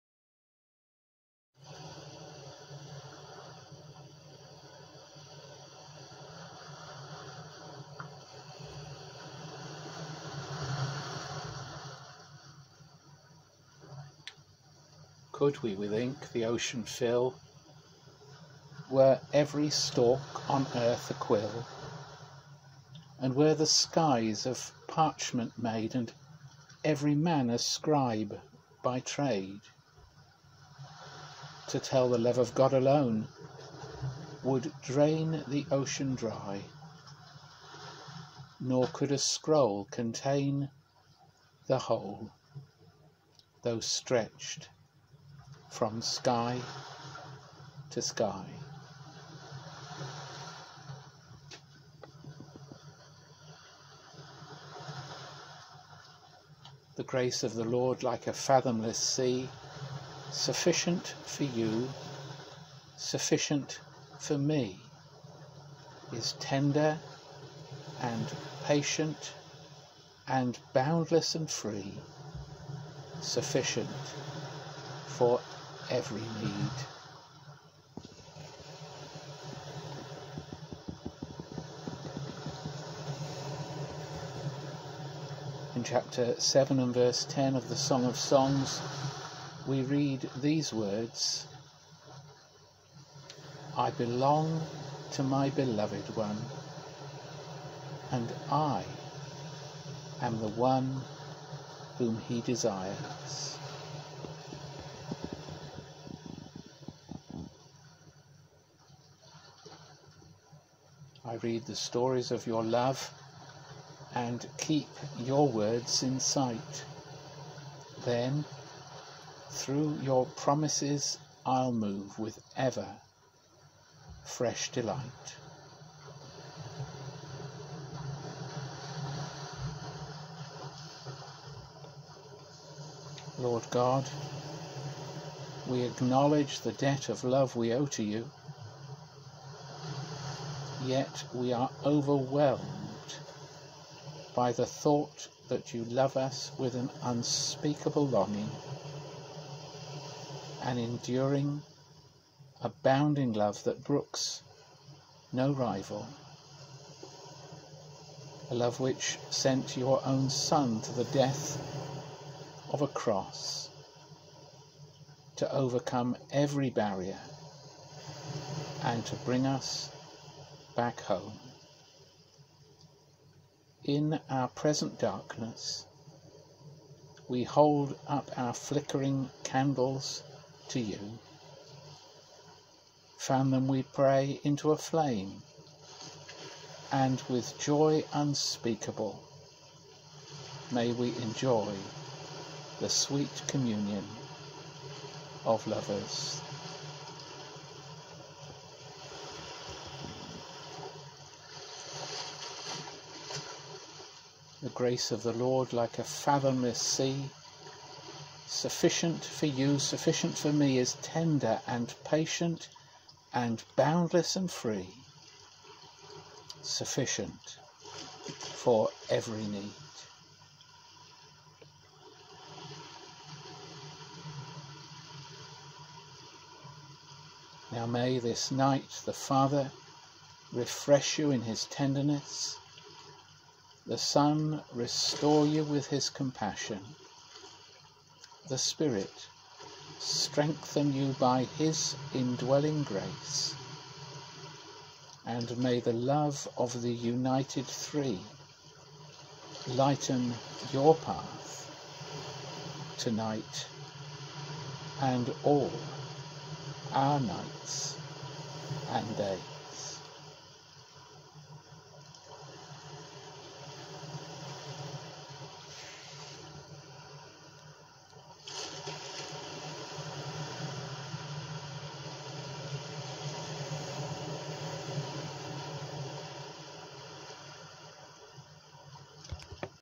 Night Prayer 11th August 2020.
A short devotion led by Stephen Dray.